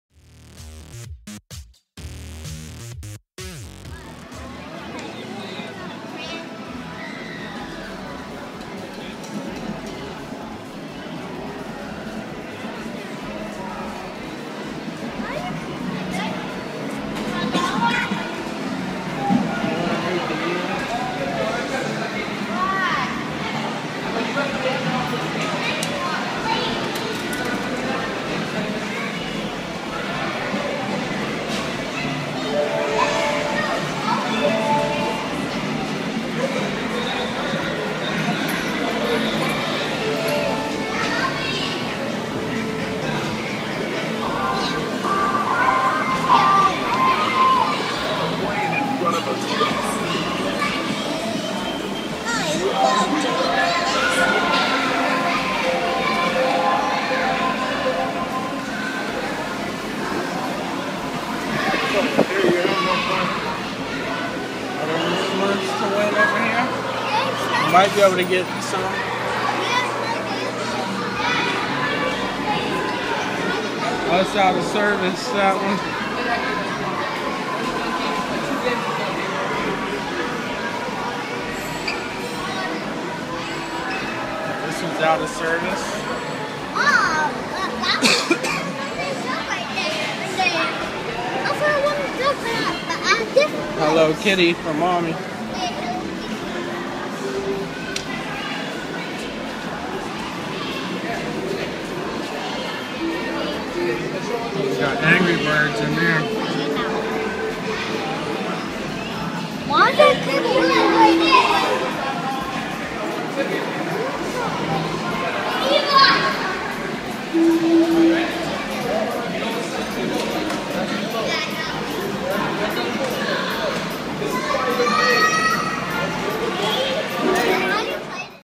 A Quick Tour of Hershey Park's other Arcade
Arcade Adventures Episode 31

Check out more arcade games in this video walk through of the second arcade found at Hershey Park.